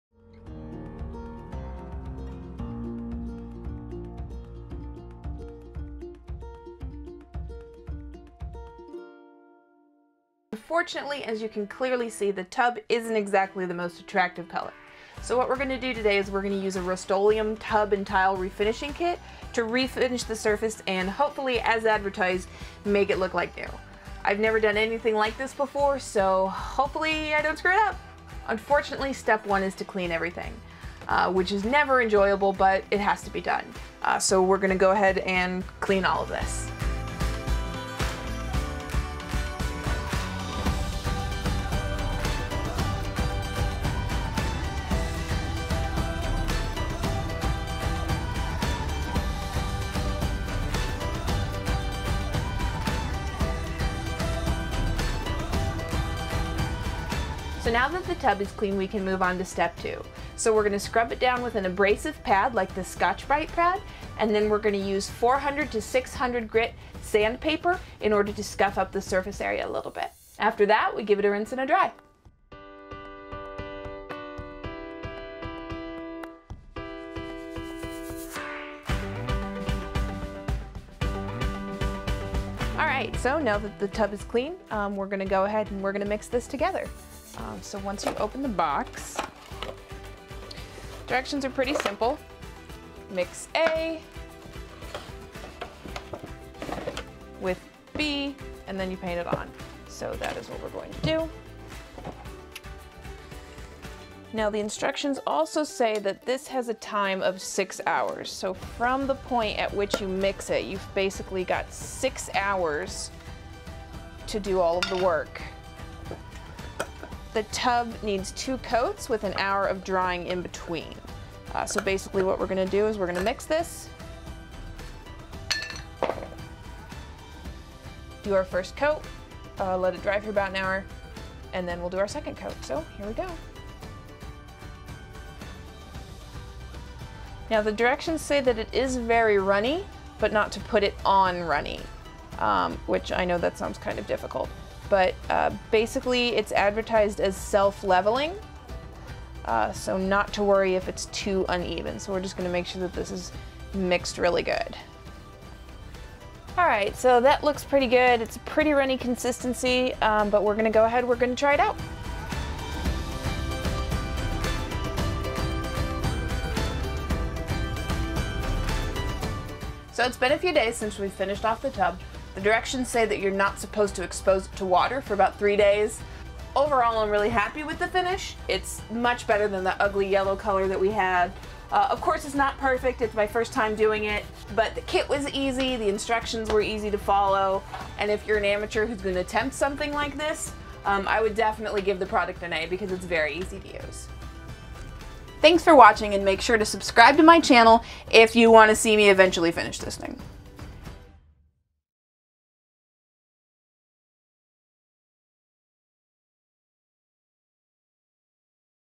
Travel Trailer Remodel, Part 6: Refinishing the Tub with Rust-Oleum Tub Kit
One veteran, one trailer, and what I think are a bunch of tools.

I refinished the tub using Rust-Oleum Tub and Tile Refinishing Kit. It was an ugly yellow color - now it is not!

Also, I've removed the longer intro. If you're interested in seeing it check out the previous video.